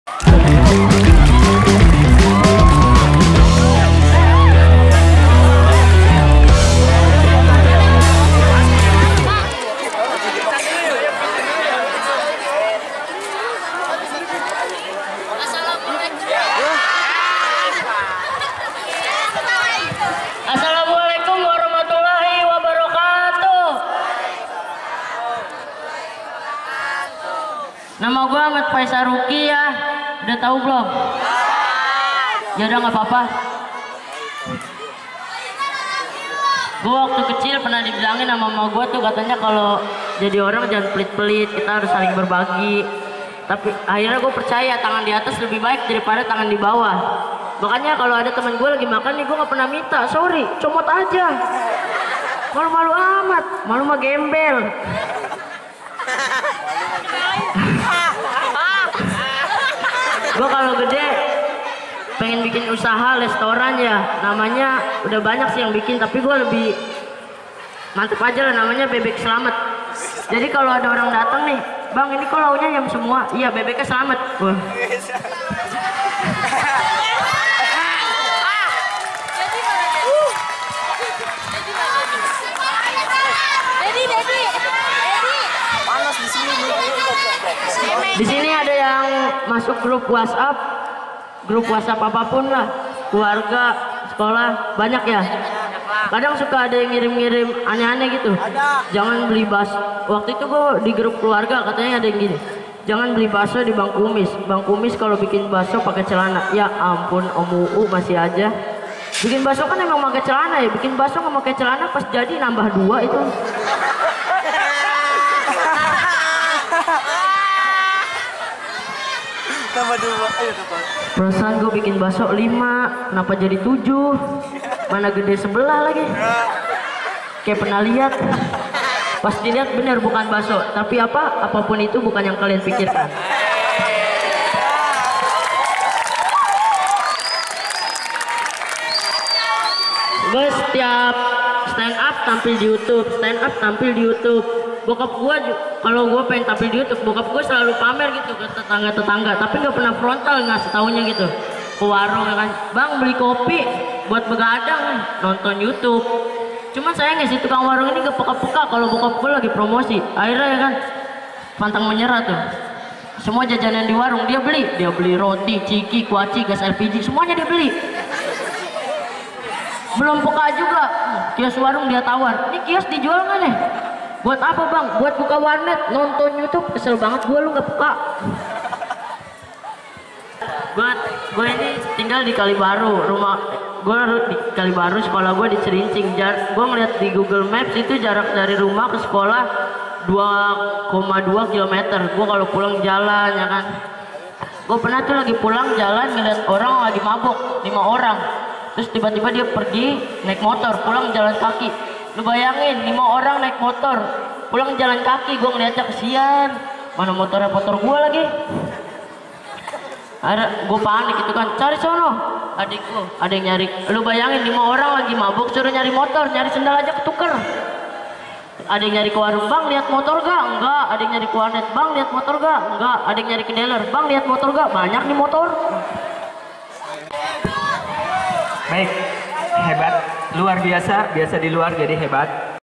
STAND UP dadakan Siapa Takut
STAND UP dadakan Faisal Ruki SMP Syahid 1 Jakarta